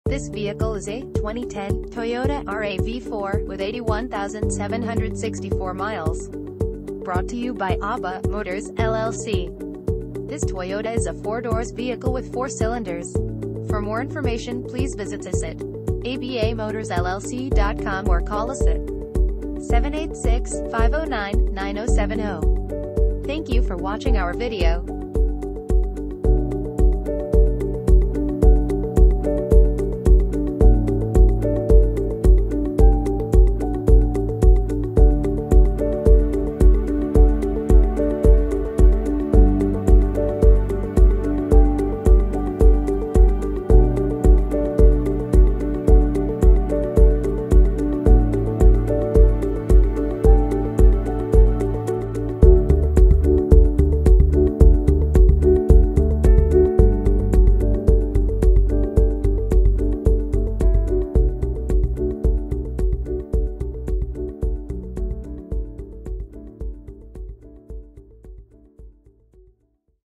2010 Toyota RAV4 - ABA MOTORS LLC
ABA MOTORS LLC
10430 Sw 186th Street

2010 Toyota  RAV4

One owner vehicle, Clean title, No Warranty, 81,764 miles, Automatic, Pacific Blue Metallic exterior, SUV, FWD: Front Wheel Drive, 4 doors, 4 cylinders, Gasoline, MPG City 22 and 28 in Hwy.